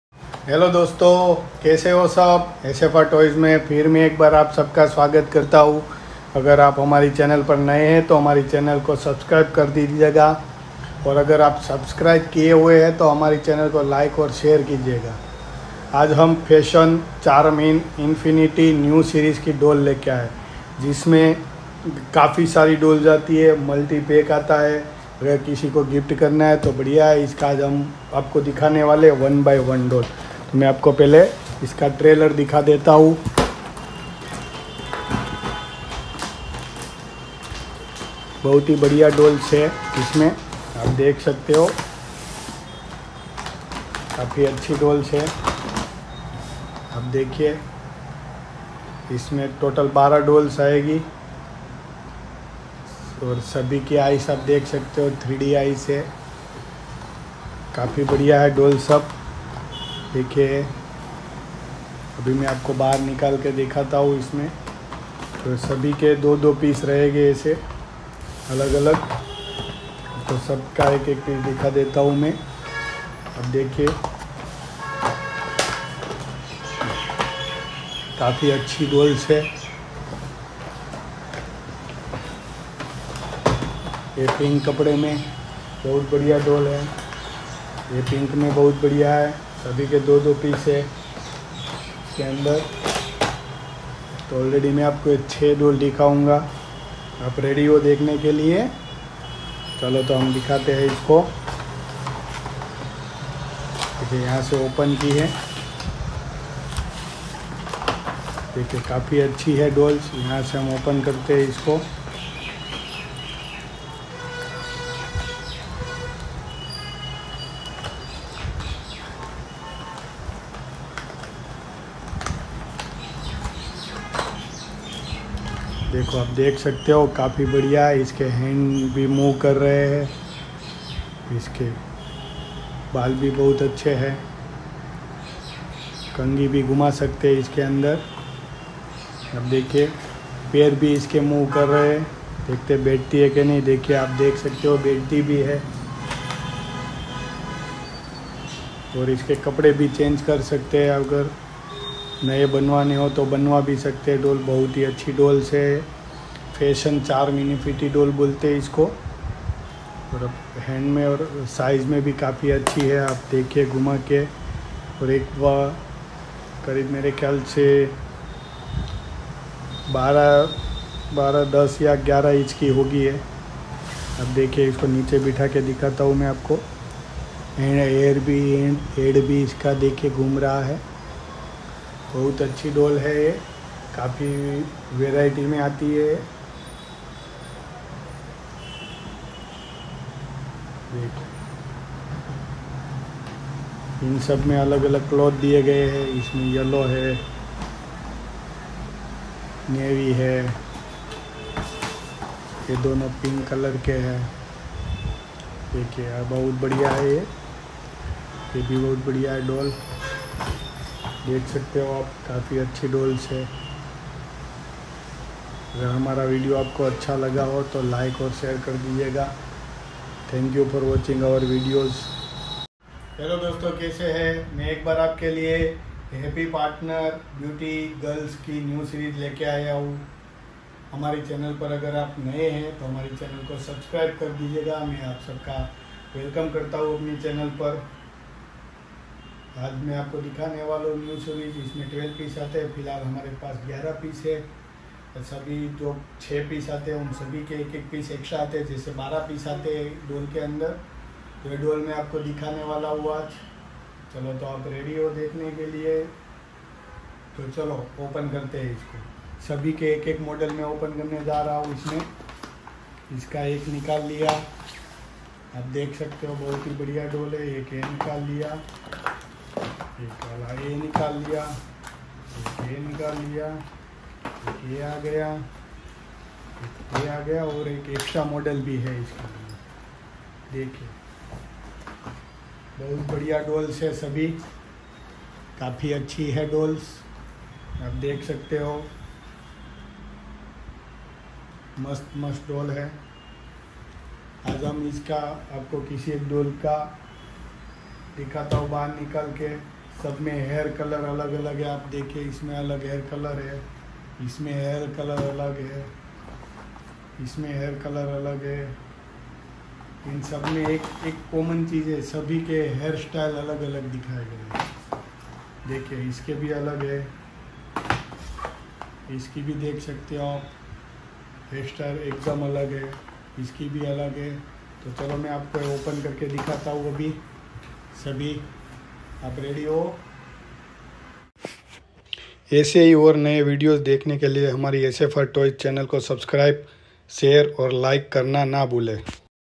Fashion Charm Infinity Doll | Cute Doll | Beautiful Doll | Princess Doll Set SFR TOYS
a sabhi ne  different type cloth pahne huve hey sabhi doll unique or diffrent hey